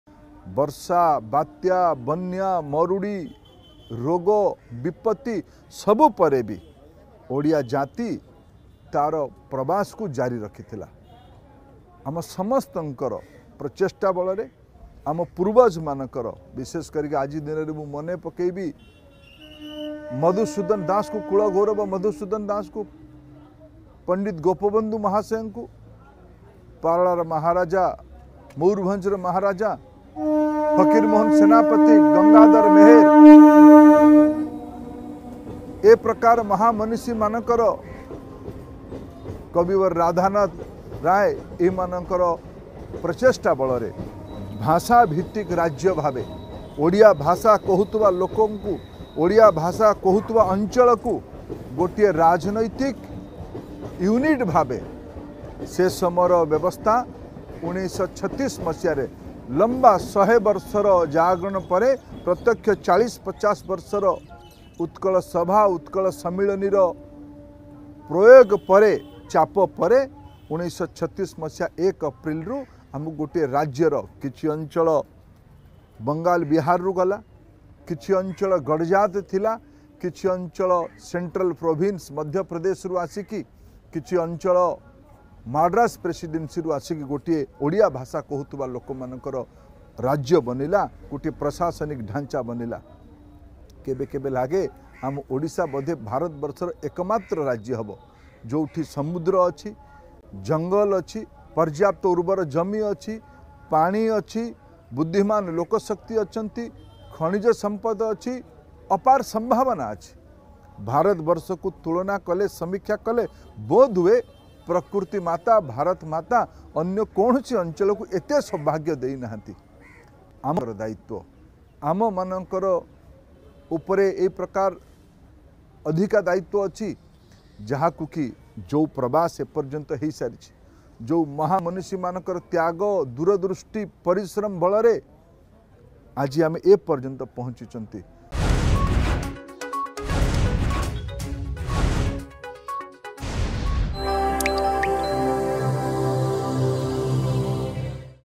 Union Minister Dharmendra Pradhan reminded the dignitaries of Odisha
ଓଡିଶାର ମହାମନିଷୀଙ୍କୁ ମନେ ପକାଇଲେ କେନ୍ଦ୍ର ମନ୍ତ୍ରୀ ଧର୍ମେନ୍ଦ୍ର ପ୍ରଧାନ
ଓଡିଶାର ସମ୍ବଳ ଅନ୍ୟ ଦେଶ ତୁଳନାରେ ବହୁତ ଅଧିକ ବୋଲି କହିଛନ୍ତି ଧର୍ମେନ୍ଦ୍ର ପ୍ରଧାନ...